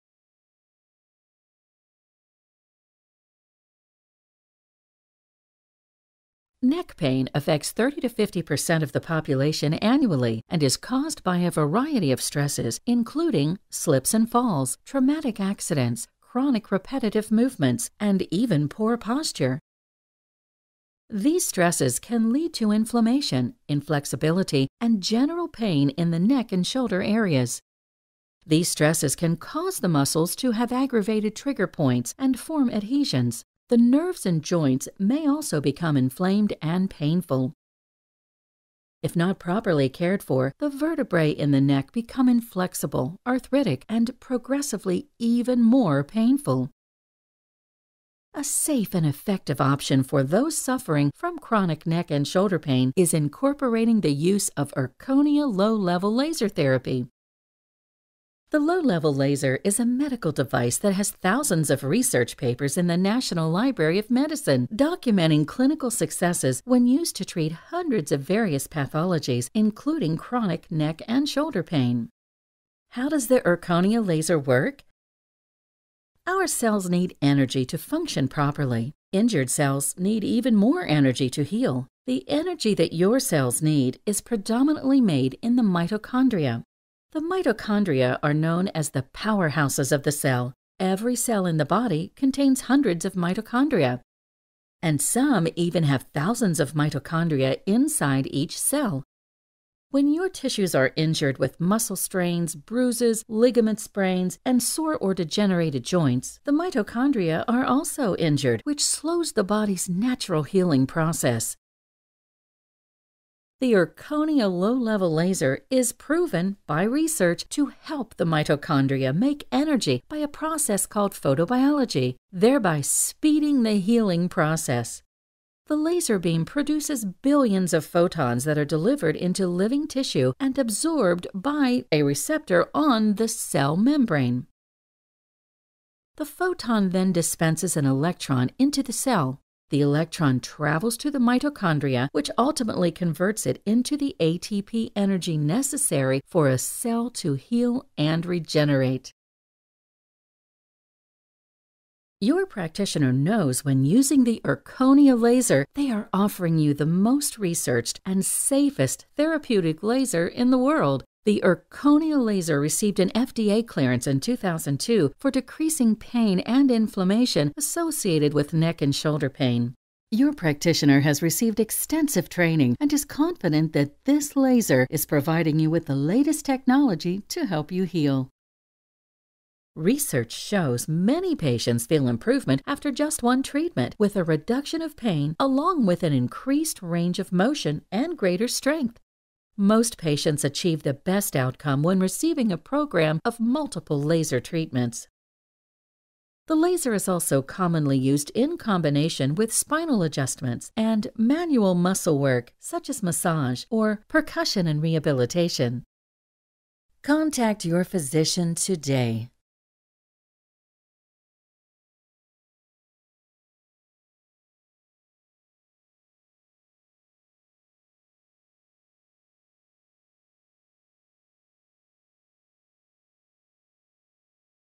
How the Erchonia Low Level Laser Helps Treat Neck and Shoulder Pain
LLLT Patient Education prepared for Dakota Back and Neck Clinic. Like this video? Shop our Customized Patient Education Videos and LLLT Waiting Room Videos